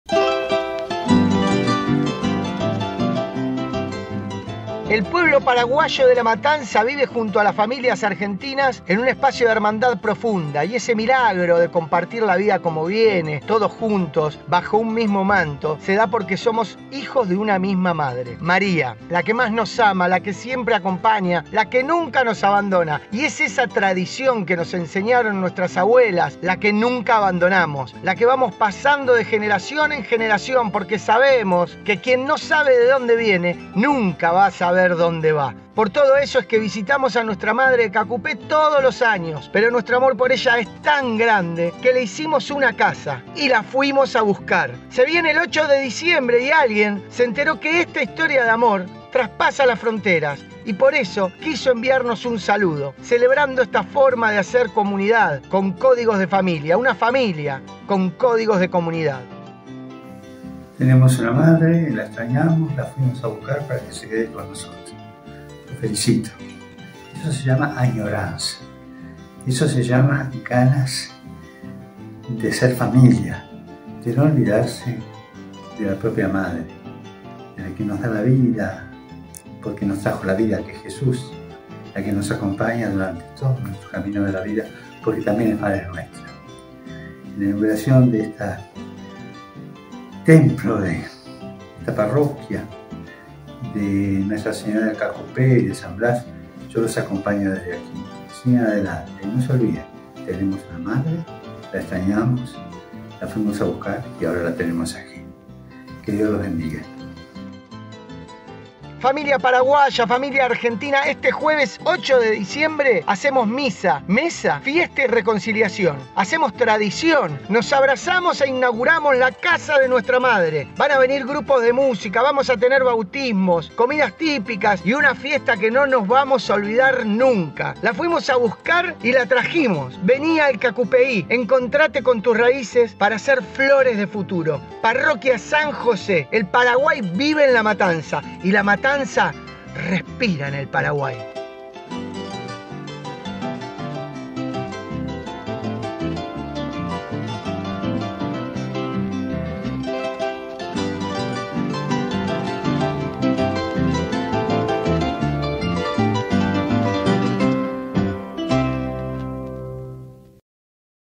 El Papa Francisco le habló a La Matanza
Tenemos Madre. La extrañamos, le hicimos una casa nueva y la fuimos a buscar.
Este 8 de diciembre, haremos la inauguración de la Capilla de la Virgen de los Milagros de Caacupé y San Blas. Estará ubicada en Av. Crovara y Cátulo Castillo, Ciudad Evita. Este será el lugar que reciba a sus hijos dispersos, para que sientan el abrazo materno.

Ese día comenzaremos celebrando la misa en guaraní a las 8:00 hs., a las 9:00 hs. bautismos populares. Luego, a las 14 hs. iniciará la feria de emprendedores con comidas típicas, calesita, shows y juegos infantiles.
A las 17:00 hs. celebraremos la misa con el Obispo Mons. Eduardo García, con Comunión y Confirmación de adultos.
A su término, continuaremos con el festival popular animado por Derlis Gerardo, El Tucu Marlia y El Trovador.